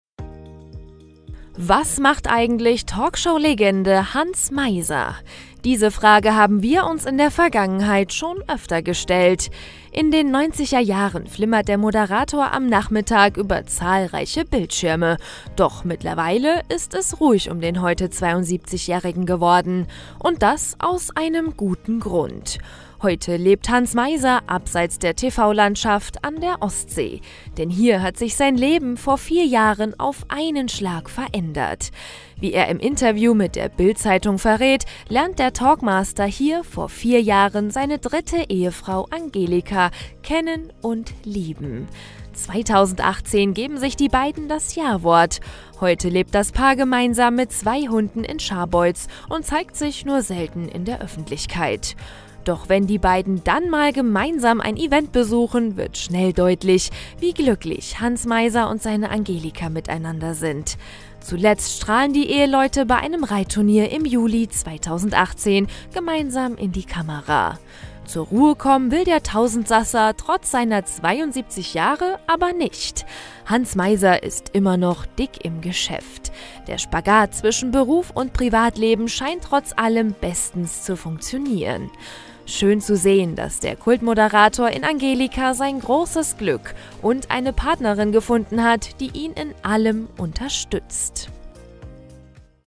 Hans Meiser - Seit einem Jahr verheiratet: Jetzt zeigt der Ex-RTL-Talkmaster uns seine Angelika
Was macht eigentlich Talkshow-Legende Hans Meiser? In den 90er-Jahren flimmert der Moderator am Nachmittag über zahlreiche Bildschirme, doch mittlerweile ist es ruhig um den heute 72-Jährigen geworden